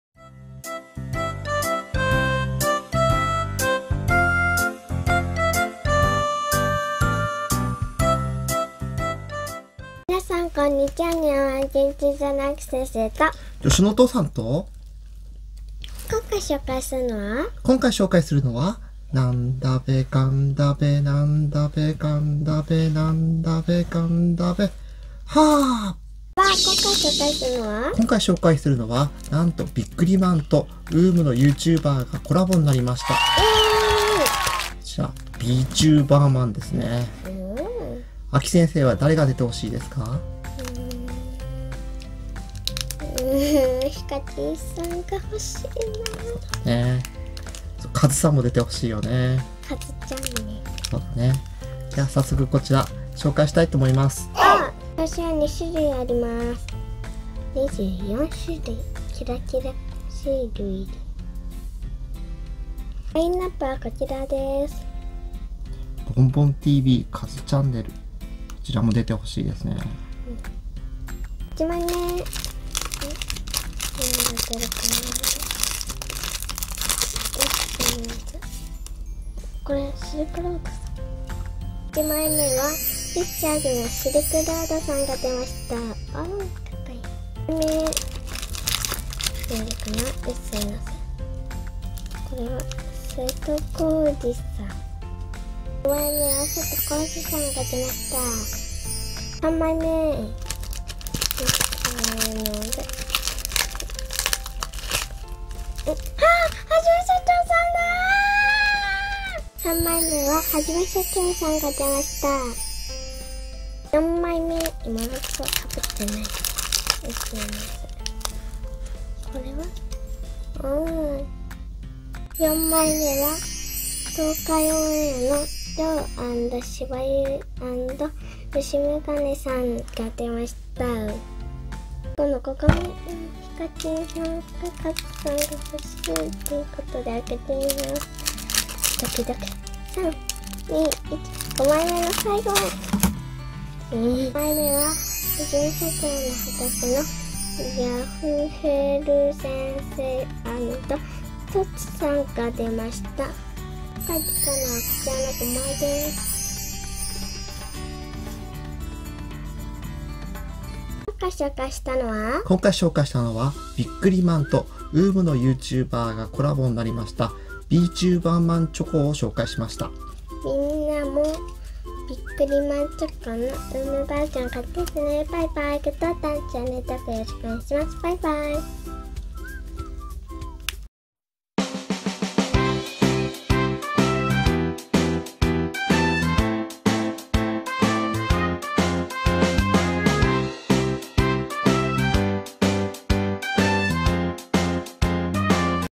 Bチューバーマン チョコ開封します。
なんと、UUUMの有名YouTuberがビックリマンとコラボ。
ビックリマン大好き。
そしてUUUMYouTuber大好き。
ということで早速購入しました。

はたしてアキ先生が好きなYouTuberは登場するのでしょうか？